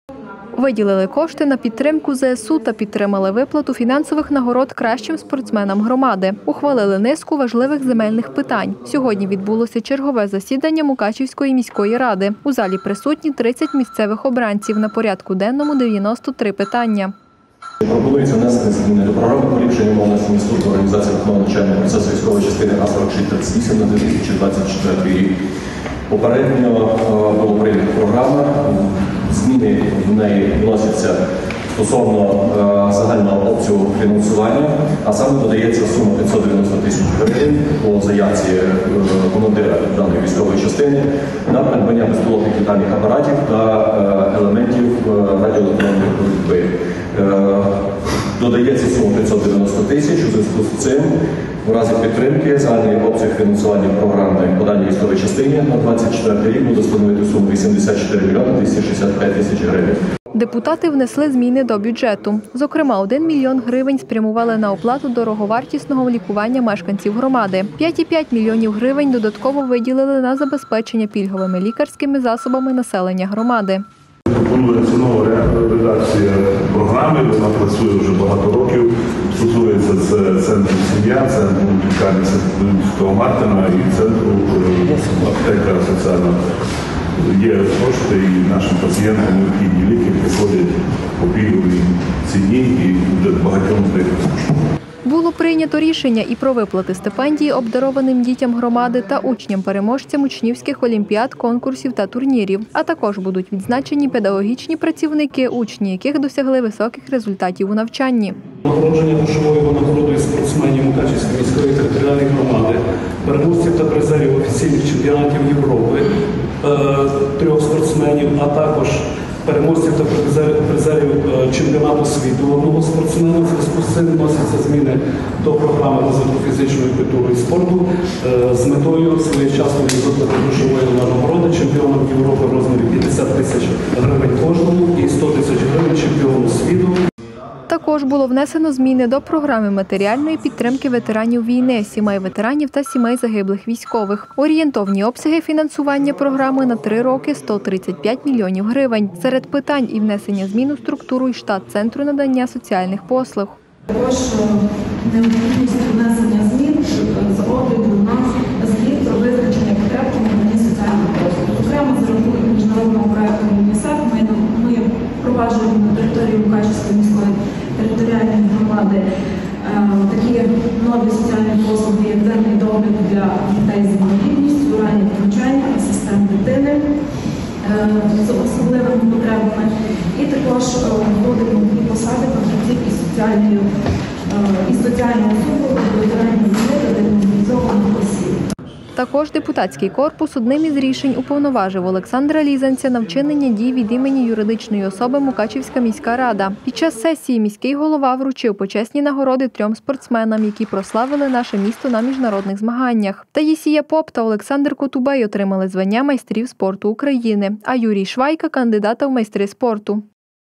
Важливі рішення
Виділили кошти на підтримку ЗСУ та підтримали виплату фінансових нагород кращим спортсменам громади. Ухвалили низку важливих земельних питань. Сьогодні відбулося чергове засідання сесії Мукачівської міської ради. У залі присутні 30 місцевих обранців на порядку денному 93 питання. Депутати внесли зміни до бюджету. Зокрема, 1 млн грн – спрямували на оплату дороговартісного лікування мешканців міста;  5,5 млн грн – додатково виділили на забезпечення пільговими лікарськими засобами населення громади. Було прийнято рішення і про виплати стипендій обдарованим дітям громади та учням-переможцям учнівських олімпіад, конкурсів та турнірів. А також будуть відзначені педагогічні працівники, учні яких досягли високих результатів у навчанні. Також було внесено зміни до Програми матеріальної підтримки ветеранів війни, сімей ветеранів та сімей загиблих військових. Орієнтовні обсяги фінансування програми на 3 роки – 135 млн гривень. Серед питань і  внесення змін у структуру і штат Центру надання соціальних послуг. Також Депутатський корпус одним із рішень уповноважив Олександра Лізанця на вчинення дій від імені юридичної особи Мукачівська міська рада. Під час сесії міський голова вручив почесні нагороди трьом спортсменам, які прославили наше місто на міжнародних змаганнях. Таїсія Поп та Олександр Котубей отримали звання майстрів спорту України, а Юрій Швайка – кандидата в майстри спорту.